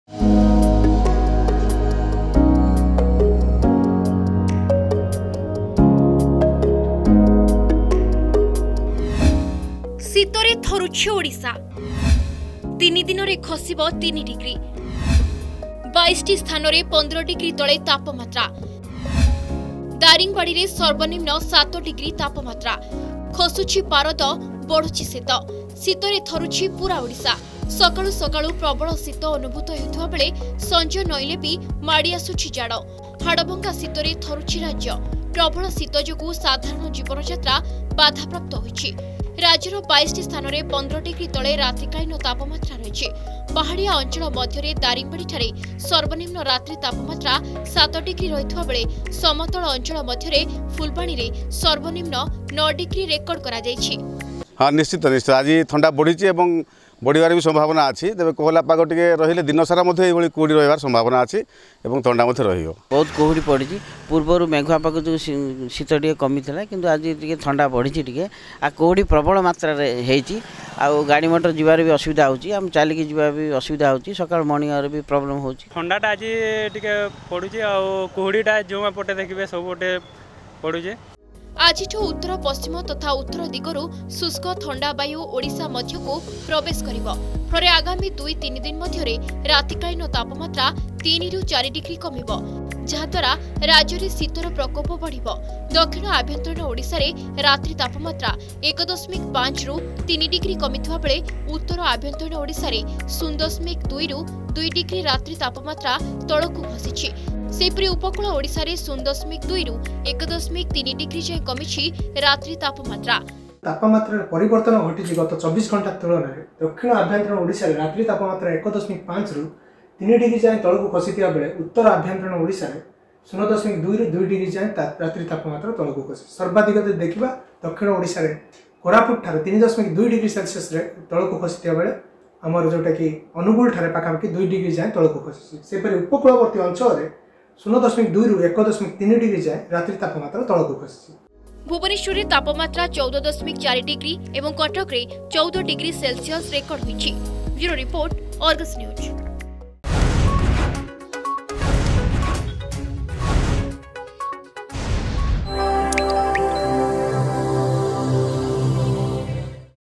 ଶୀତରେ ଥରିବ ଓଡ଼ିଶା, ୩ଦିନରେ ଖସିବ ୩ଡିଗ୍ରୀ
ଖସୁଛି ପାରଦ, ବଢୁଛି ଶୀତ। ଶୀତରେ ଥରୁଛି ପୂରା ଓଡ଼ିଶା। ସକାଳୁ ସକାଳୁ  ପ୍ରବଳ ଶୀତ ଅନୁଭୂତ ହେଉଥିବା ବେଳେ ସଞ୍ଜ ନଇଁଲେ ବି ମାଡିଆସୁଛି ଜାଡ। ହାଡଭଙ୍ଗା ଶୀତରେ ଥରୁଛି ରାଜ୍ୟ। ପ୍ରବଳ ଶୀତ ଯୋଗୁ ସାଧାରଣ ଜୀବନଯାତ୍ରା ବାଧାପ୍ରାପ୍ତ ହୋଇଛି। ରାଜ୍ୟର ୨୨ଟି ସ୍ଥାନରେ ୧୫ଡ଼ିଗ୍ରୀ ତଳେ ରାତ୍ରିକାଳୀନ ତାପମାତ୍ରା ରହିଛି।